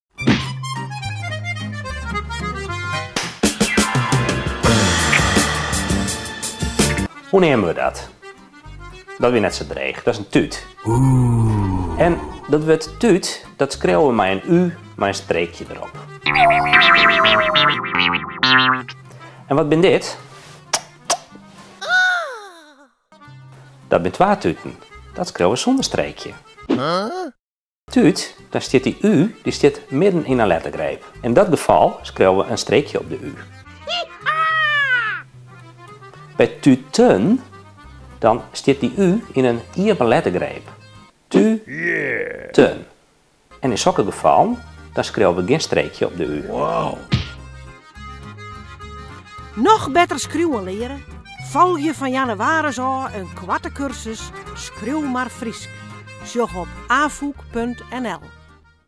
Skriuw mar Frysk: mei in tútsje fan... Henk?
Praat mar Frysk lansjearret fan no moandei 22 oktober ôf oan spesjaal foar al ús Facebook-, Hyves- en Twitterfreonen in kursus Frysk skriuwen! Hielendal fergees! Us taalprofessor Henk sil yn in 14 lessen fia social media it Frysk skriuwen in bytsje bybringe. Leuk no?

Wannear brûkst no eins in 'ch' en wannear in 'g' yn it Frysk? En is it no 'tûtsje' of 'tútsje'? Us Henk leit jim presys út hoe dat allegear yn it Frysk wurket. Boppedat, foar de échte studinten ûnder jim, hâlde wy alle wiken op de tongersdeis in diktee. Dan kinst moai tsjekke oftst de stof in bytsje machtich bist!

Alfêst yn 'e kunde komme mei ús Henk? Tsjek it filmke! En graach oant moandei 22 oktober! Do dochst dochs ek mei?